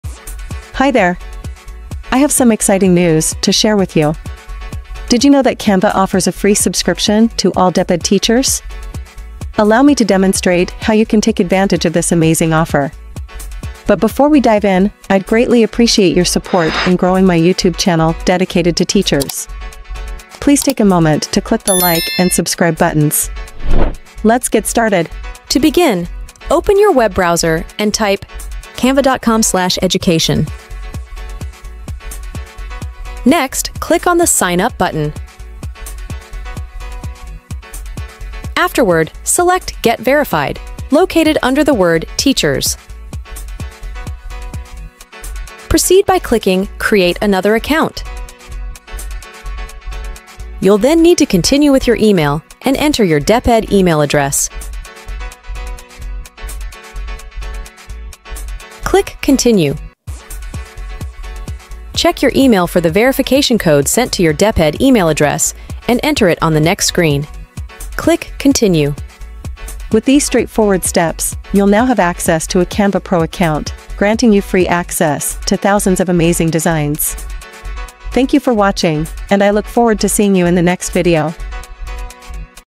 HOW TO AVAIL FREE PREMIUM CANVA ACCOUNT FOR DEPED TEACHERS
Tired of limited features in your Canva free account?

As a DepEd teacher, you can unlock Canva Pro's amazing tools and resources for FREE!

In this video, I'll walk you through the step-by-step process on how to claim your DepEd Canva Pro account.

Here's what you'll learn:

Eligibility requirements for DepEd teachers
Different ways to verify your DepEd status
Step-by-step guide on claiming your Canva Pro account
Tips and tricks for getting the most out of Canva Pro
No matter your tech experience, this video will make it easy for you to claim your Canva Pro account and start creating stunning visuals for your classroom.

Don't miss out on this awesome opportunity!

Here are some additional resources that you may find helpful:

Let's create amazing things together!